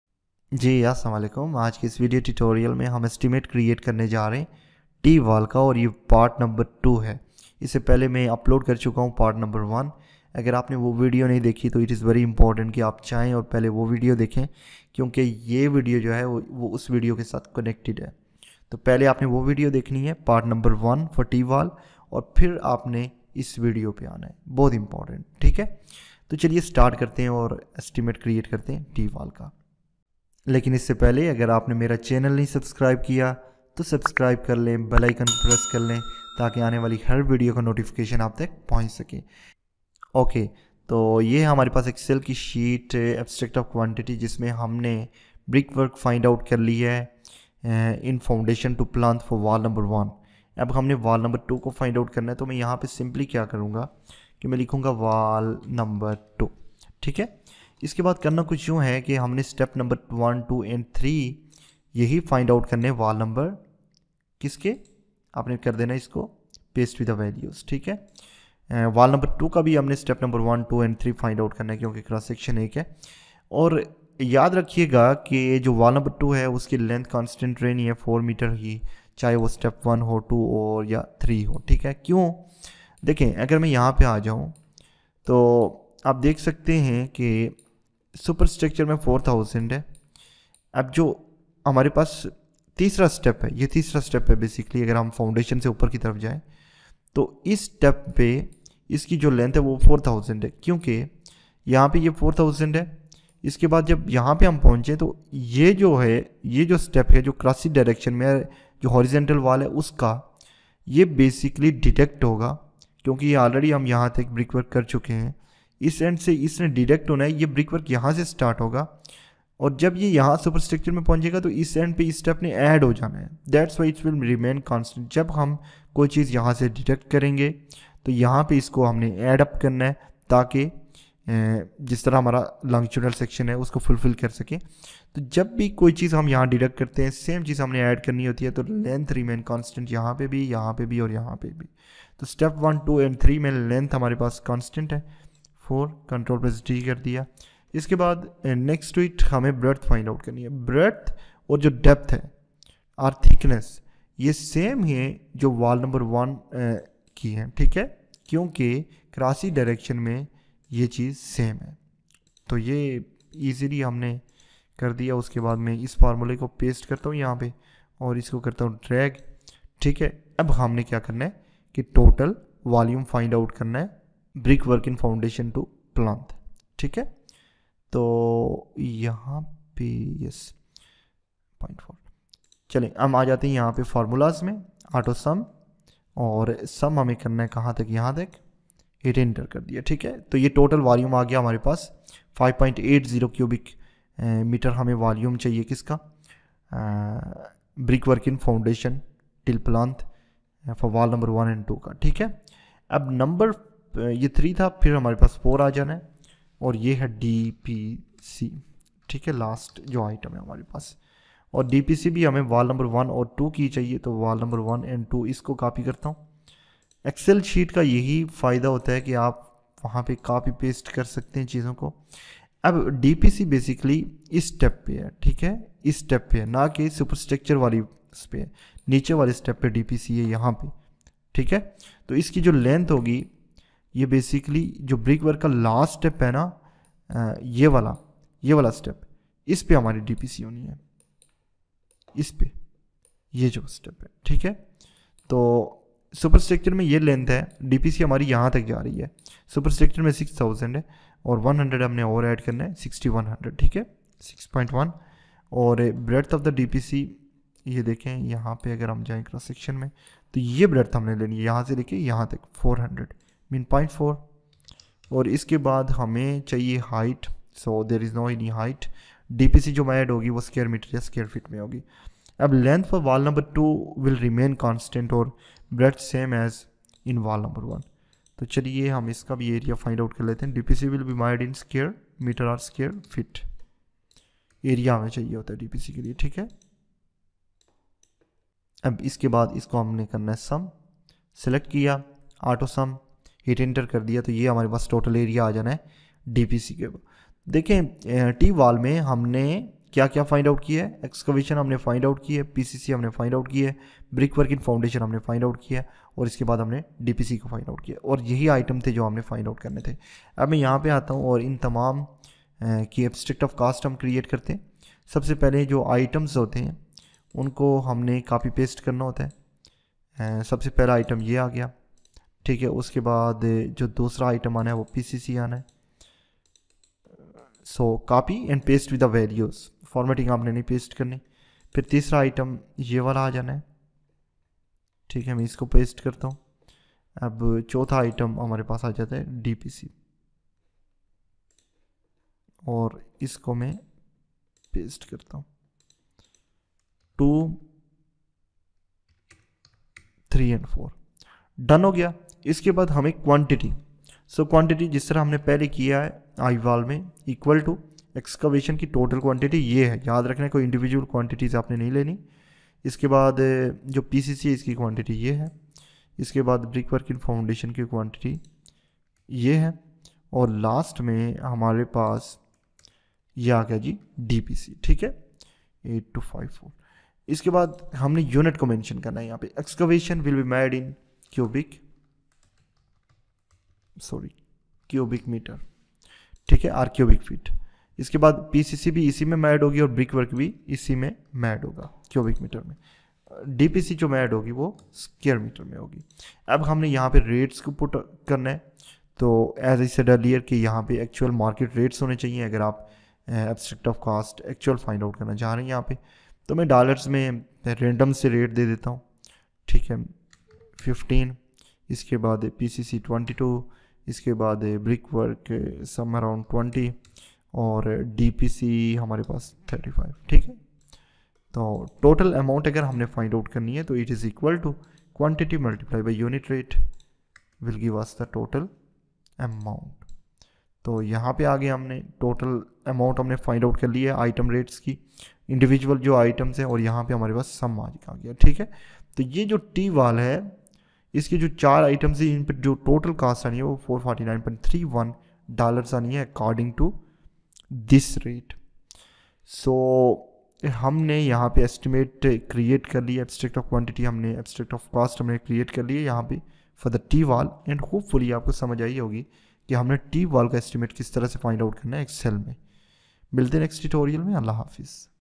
Estimating And Costing | Estimate For T Wall | Long Wall Short Wall Method.اردو/ हिंदी
Estimating And Costing ? Estimate For I Wall ? Long Wall Short Wall Method?
In this video, we will focus on the estimation of T walls in civil engineering and how to create an estimate for them using Excel. This is Part 2 of the series, where we will cover the basics of T walls, their design, and the data required for estimation. We will also demonstrate step-by-step how to create an estimate using Excel, making it easier for you to follow along.

Your Queries.
1.Esitmate For T Shape Wall.
2.Estimate For T Wall.
3.Detailed Estimate Of T shape Wall.
4.Long Wall And Short Wall Mehtod.